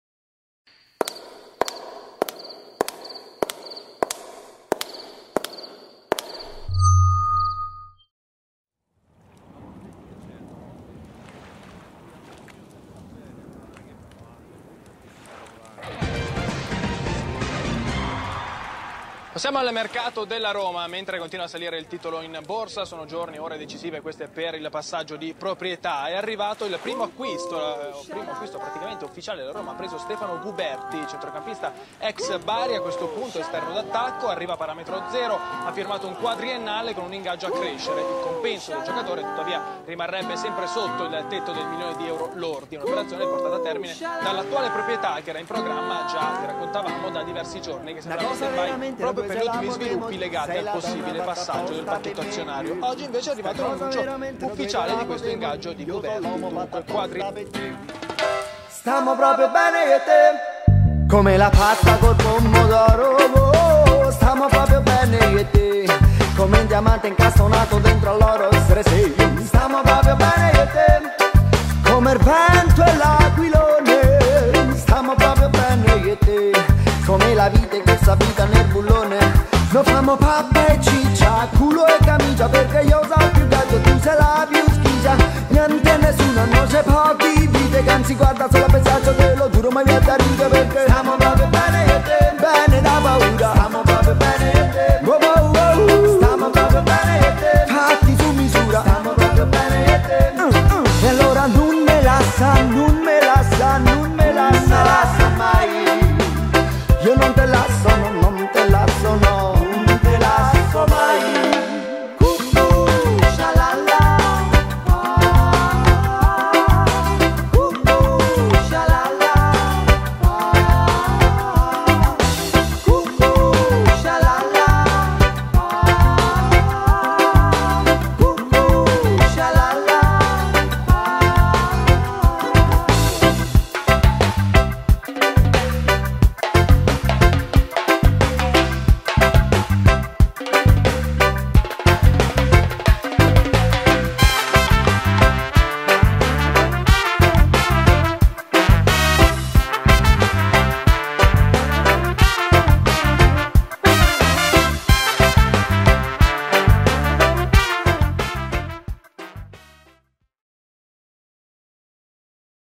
STEFANO GUBERTI ARRIVA NELLA AS ROMA
Nuovo acquisto della AS Roma. Stefano Guberti, esterno veloce e di talento, firma un quadriennale con la squadra capitolina